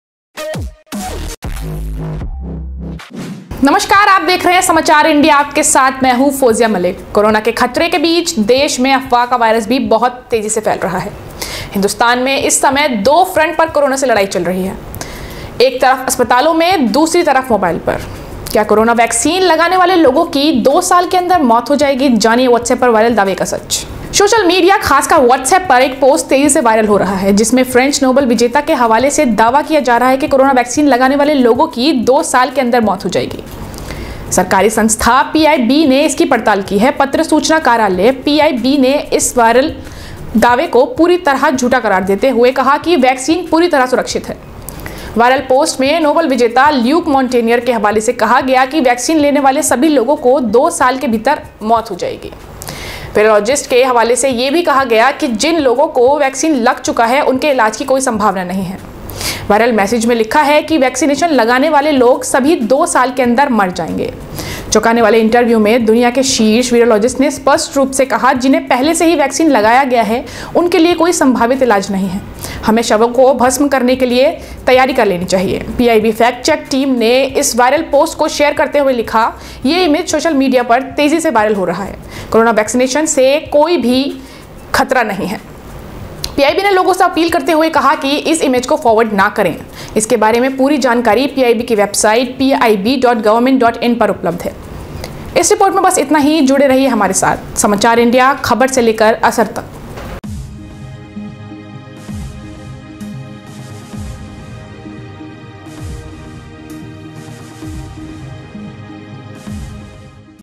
कोरोना वैक्सीन लगवाने वाले हो जाए सावधान, 2 साल मे हो सकती है आपकी मौत ? corona virus | fact check
Samachar India,News India,Bollywood,Sports,Politics || From news to effect || samacharindia,